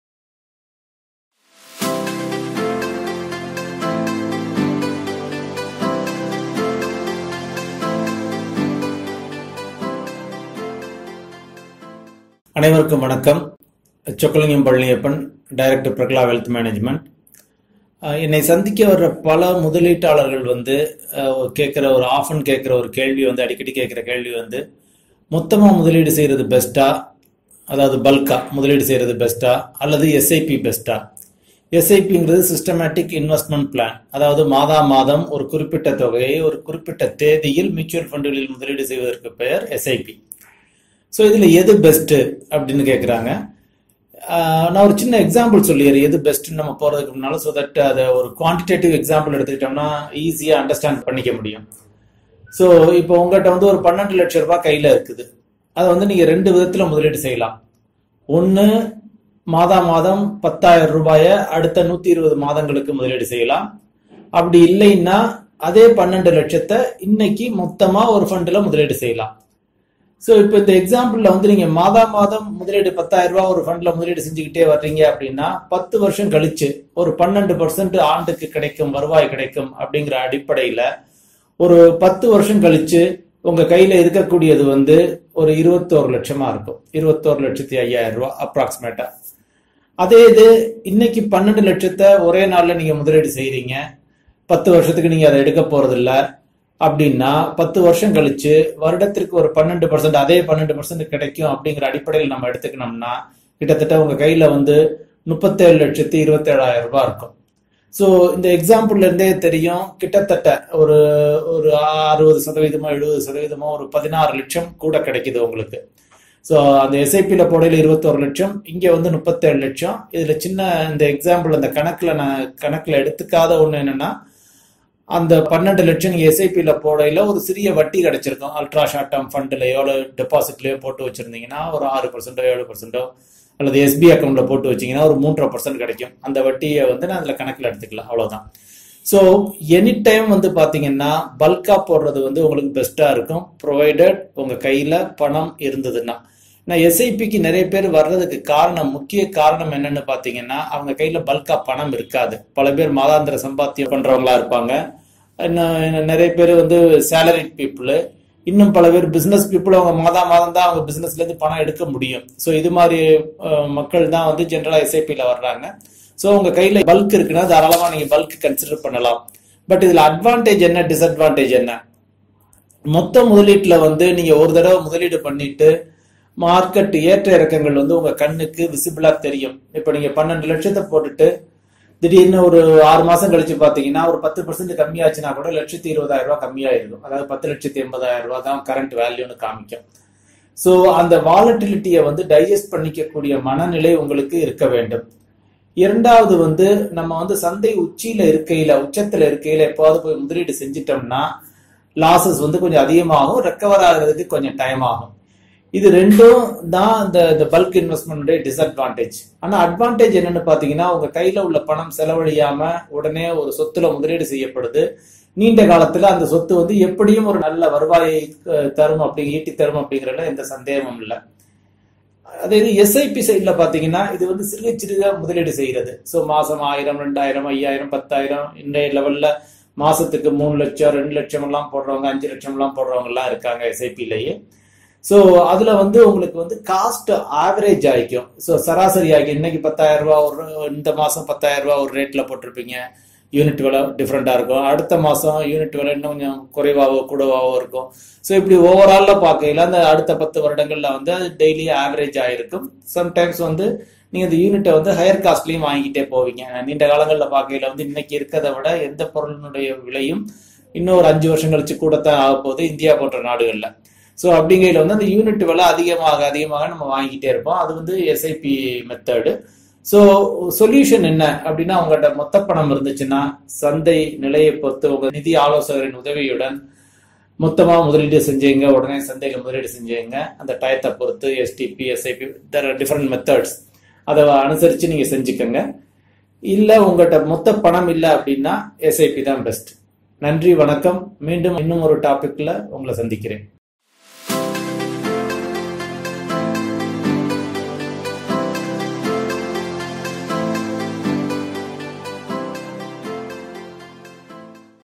SIP vs Bulk Investment In Mutual Funds (Tamil) | Prakala Wealth | By Chokkalingam Palaniappan
Click on the link above to download notes.

to know more about our service offerings.